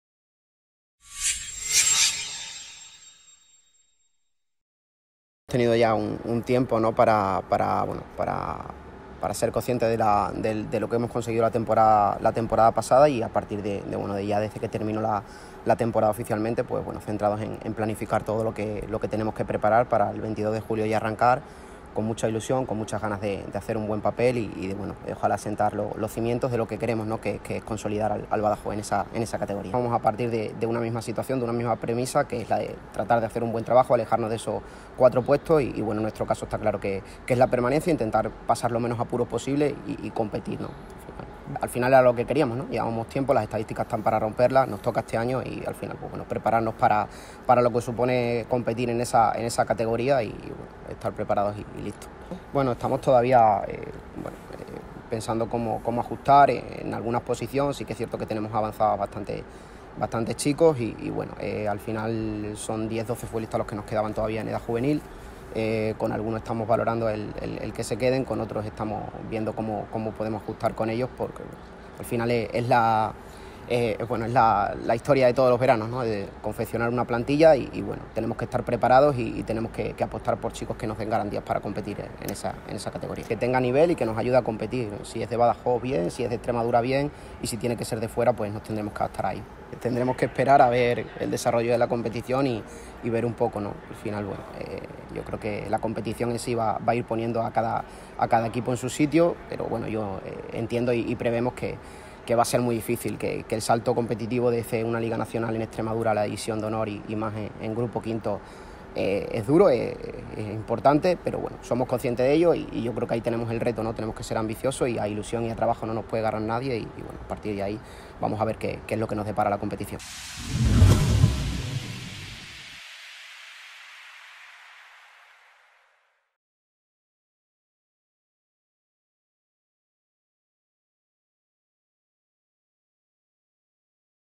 Miguel Espejo
El técnico del CD Badajoz Juvenil, Miguel Espejo, repasa la actualidad de su equipo a pocos días de comenzar una pretemporada que le va a llevar de vuelta a División de Honor Juvenil.